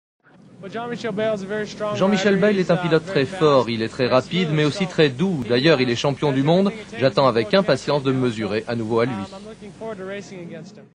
Ricky Johnson parle de Jean-Michel Bayle en 1989
Ricky Johnson parle de Jean-Michel Bayle et de son arrivée aux Etats-unis lors de ce début d'année 1989.